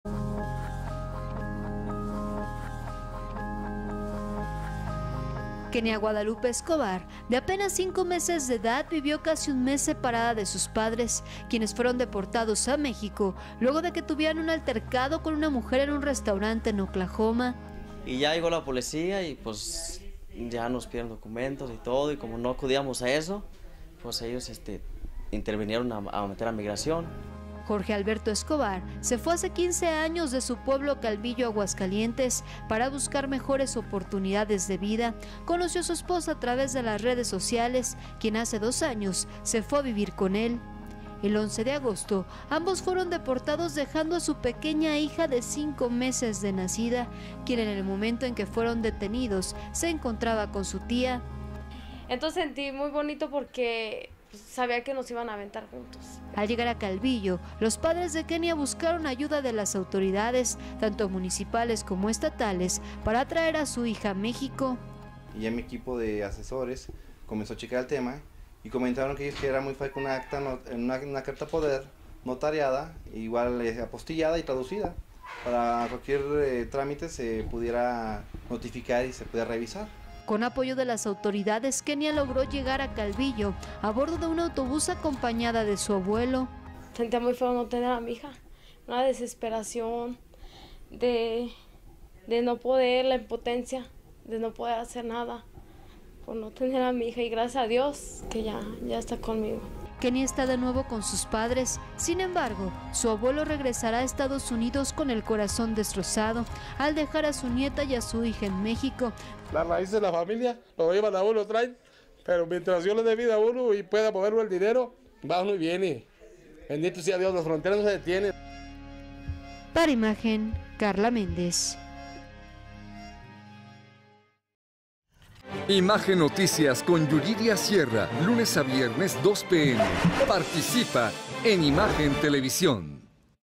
Padres son deportados y separados de su recién nacida | Noticias con Yuriria Sierra
Jorge Escobar y Yesenia Orozco estuvieron separados durante un mes de su hija recién nacida después de que fueran deportados a México tras un altercado en un restaurante en Estados Unidos.